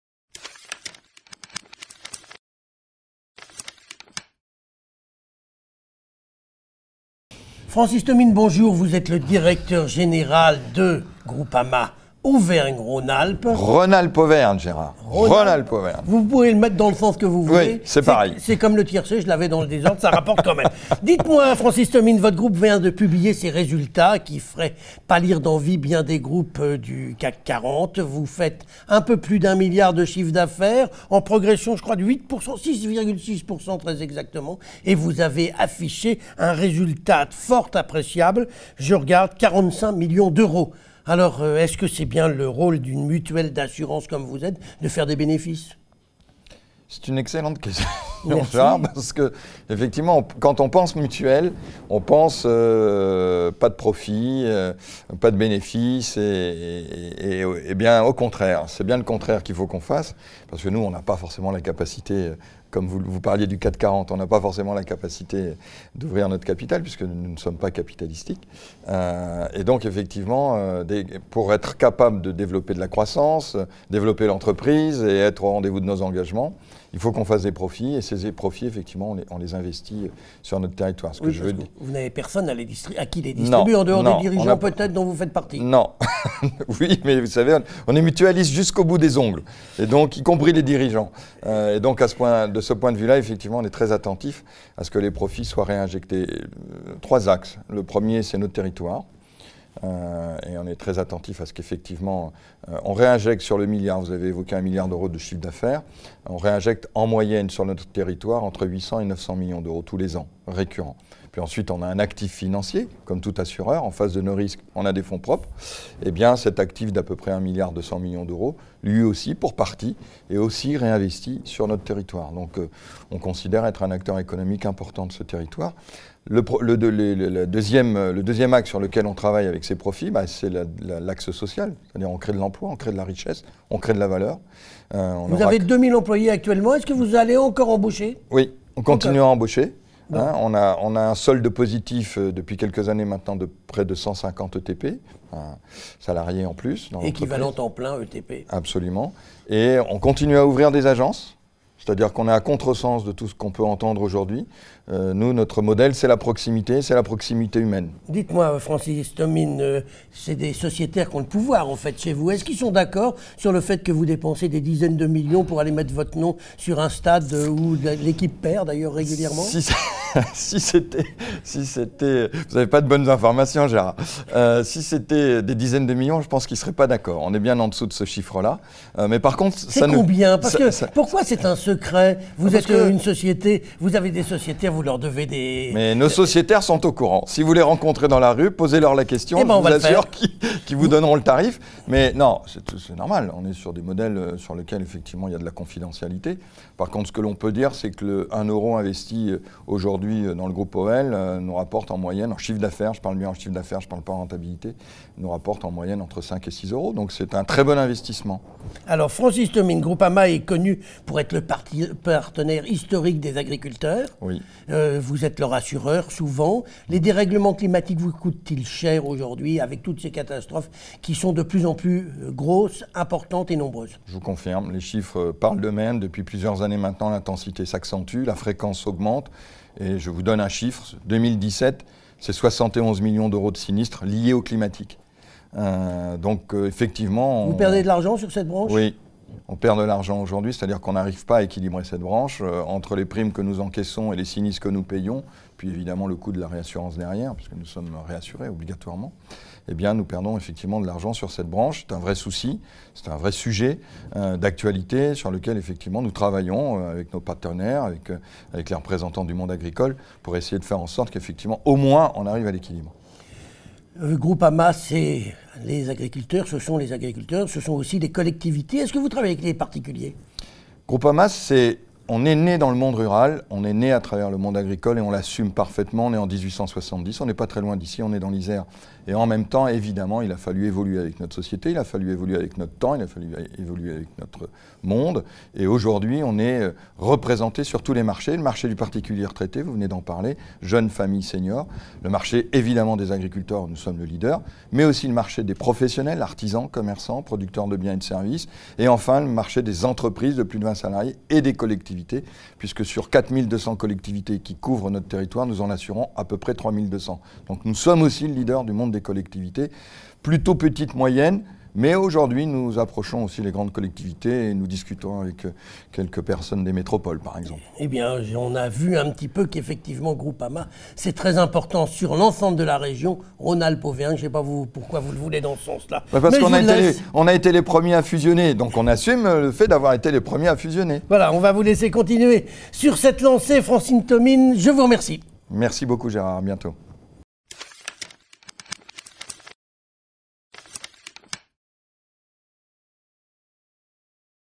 Francis Thomine (Groupama) : "Notre modèle, c'est la proximité humaine"
Le directeur général de Groupama Rhône-Alpes Auvergne, Francis Thomine, était l'invité ce jeudi matin de Gérard Angel, dans les Coulisses du Grand Lyon.